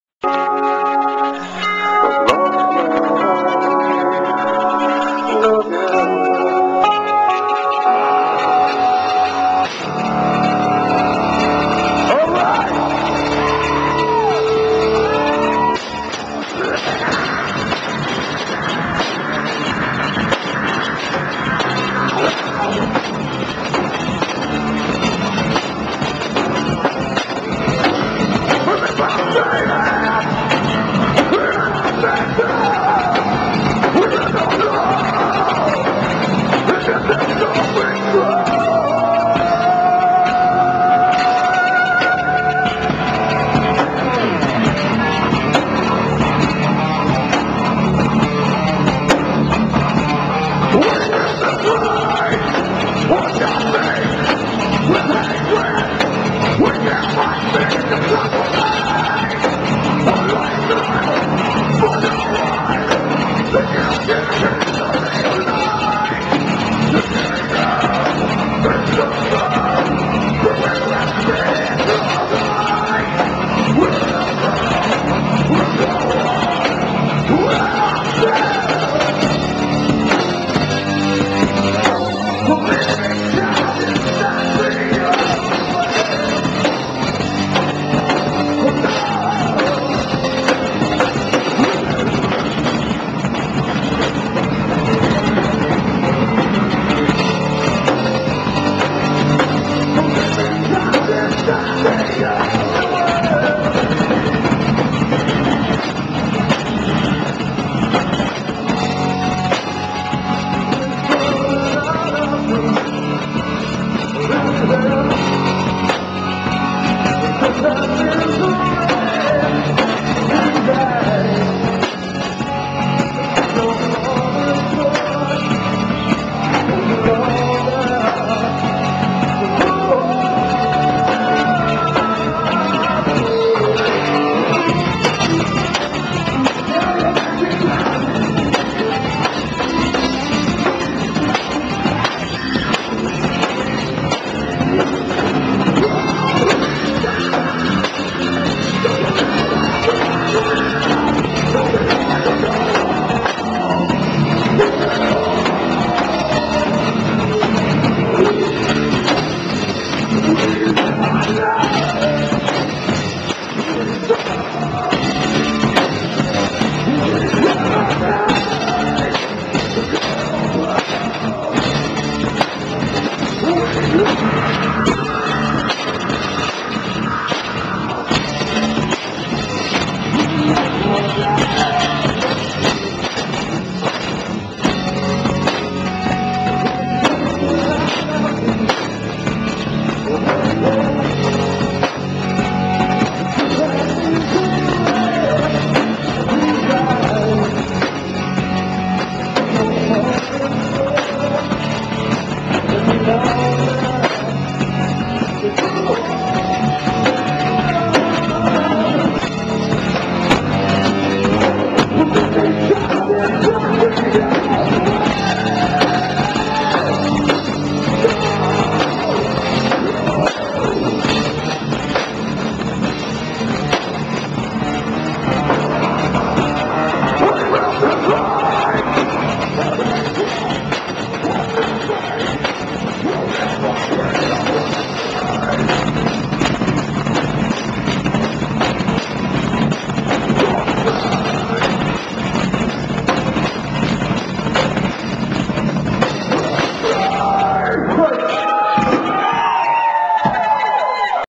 River Blindness - 99x Homegrown Live
Recorded at the Riverside Warehouse in Shreveport, LA, on July 2, 2010 using a Flip Video camcorder.